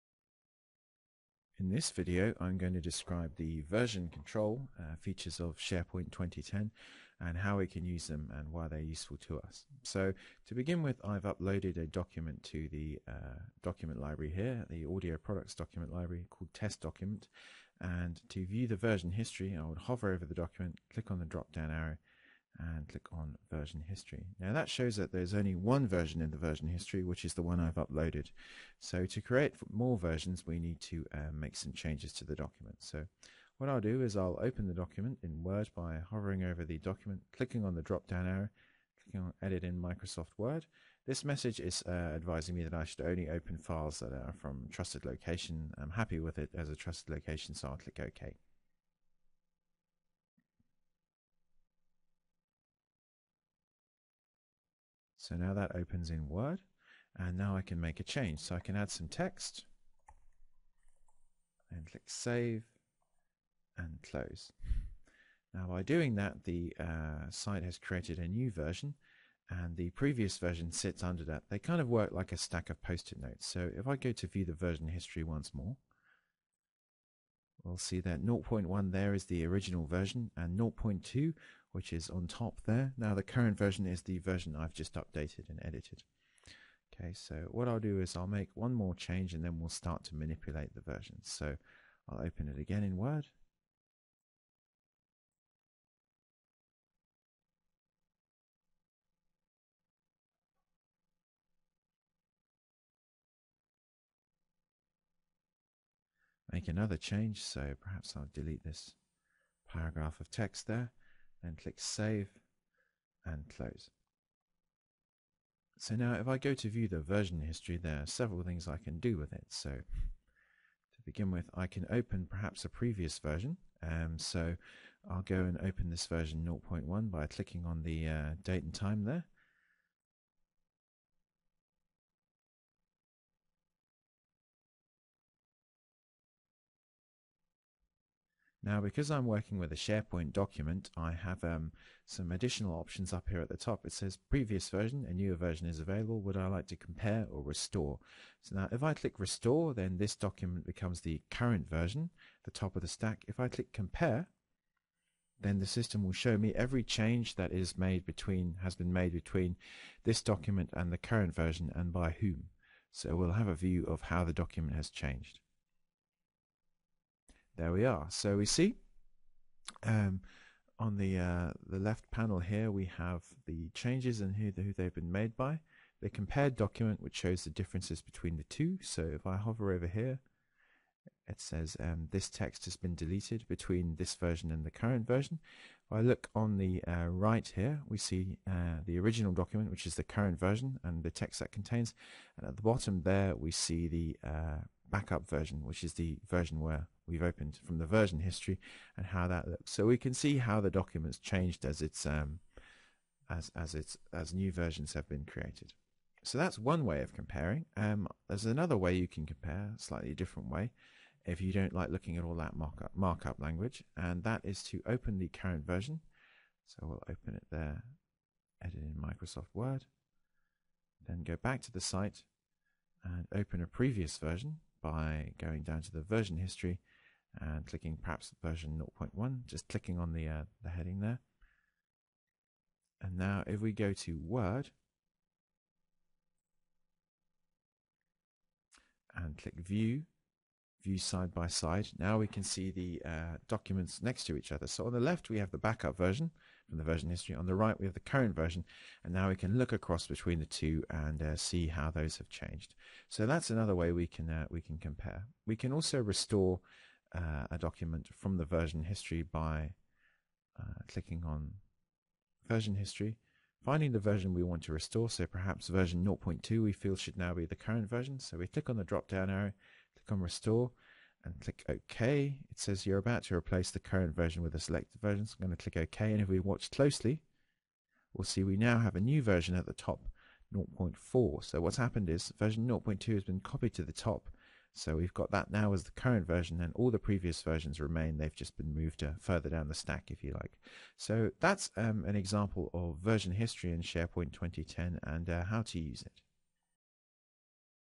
How to use Version History in SharePoint 2010
This short video covers the version control features in SharePoint 2010, why they are useful and how they can be used effectively.

Want to find out more about SharePoint 2010? to find out more.